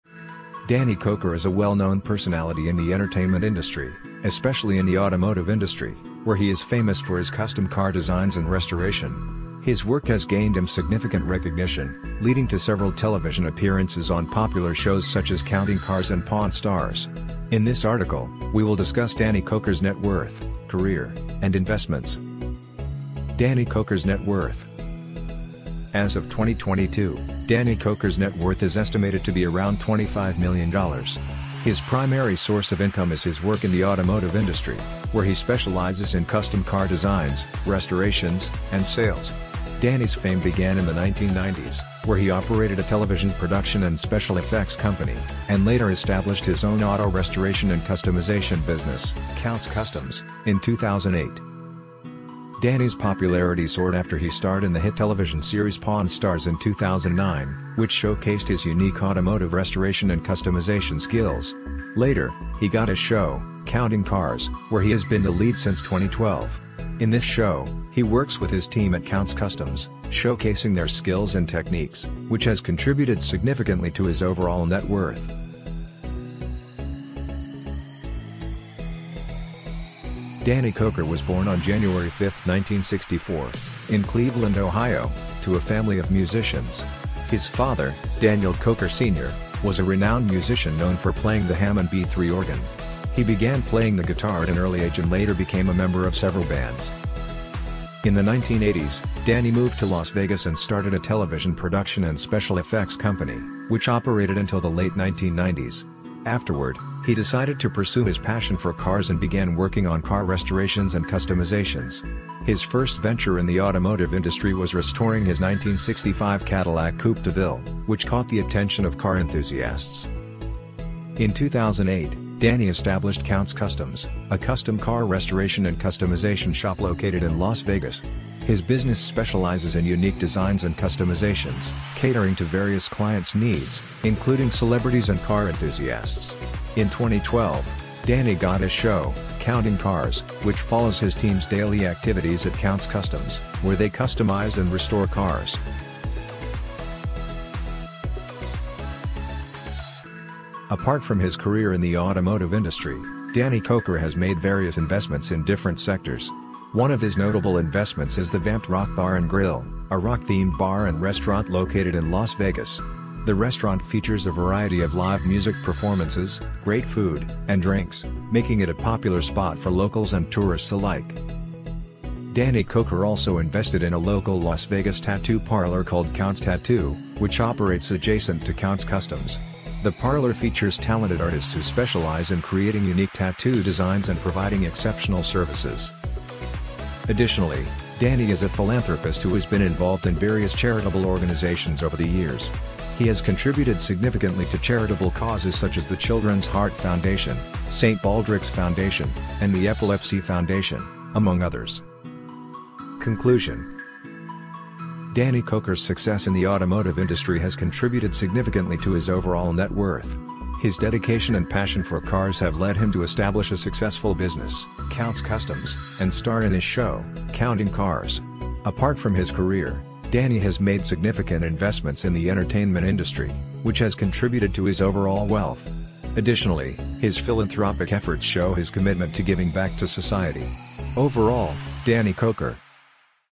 Danny Koker Lifestyle 2023 | Net Worth, Fortune, Car Collection, Mansion
Danny Koker is a well-known personality in the entertainment industry, especially in the automotive industry, where he is famous for his custom car designs and restoration. His work has gained him significant recognition, leading to several television appearances on popular shows such as "Counting Cars" and "Pawn Stars." In this article, we will discuss Danny Koker's net worth, career, and investments.

Danny Koker's Net Worth

As of 2022, Danny Koker's net worth is estimated to be around $25 million. His primary source of income is his work in the automotive industry, where he specializes in custom car designs, restorations, and sales. Danny's fame began in the 1990s, where he operated a television production and special effects company, and later established his own auto restoration and customization business, Count's Kustoms, in 2008.

Danny's popularity soared after he starred in the hit television series "Pawn Stars" in 2009, which showcased his unique automotive restoration and customization skills. Later, he got his show, "Counting Cars," where he has been the lead since 2012. In this show, he works with his team at Count's Kustoms, showcasing their skills and techniques, which has contributed significantly to his overall net worth.

Career

Danny Koker was born on January 5, 1964, in Cleveland, Ohio, to a family of musicians. His father, Daniel Koker Sr., was a renowned musician known for playing the Hammond B3 organ. He began playing the guitar at an early age and later became a member of several bands.

In the 1980s, Danny moved to Las Vegas and started a television production and special effects company, which operated until the late 1990s. Afterward, he decided to pursue his passion for cars and began working on car restorations and customizations. His first venture in the automotive industry was restoring his 1965 Cadillac Coupe de Ville, which caught the attention of car enthusiasts.

In 2008, Danny established Count's Kustoms, a custom car restoration and customization shop located in Las Vegas. His business specializes in unique designs and customizations, catering to various clients' needs, including celebrities and car enthusiasts. In 2012, Danny got his show, "Counting Cars," which follows his team's daily activities at Count's Kustoms, where they customize and restore cars.

Investments

Apart from his career in the automotive industry, Danny Koker has made various investments in different sectors. One of his notable investments is the Vamp'd Rock Bar & Grill, a rock-themed bar and restaurant located in Las Vegas. The restaurant features a variety of live music performances, great food, and drinks, making it a popular spot for locals and tourists alike.

Danny Koker also invested in a local Las Vegas tattoo parlor called "Kount's Tattoo," which operates adjacent to Count's Kustoms. The parlor features talented artists who specialize in creating unique tattoo designs and providing exceptional services.

Additionally, Danny is a philanthropist who has been involved in various charitable organizations over the years. He has contributed significantly to charitable causes such as the Children's Heart Foundation, St. Baldrick's Foundation, and the Epilepsy Foundation, among others.

Conclusion

Danny Koker's success in the automotive industry has contributed significantly to his overall net worth. His dedication and passion for cars have led him to establish a successful business, Count's Kustoms, and star in his show, "Counting Cars." Apart from his career, Danny has made significant investments in the entertainment industry, which has contributed to his overall wealth. Additionally, his philanthropic efforts show his commitment to giving back to society.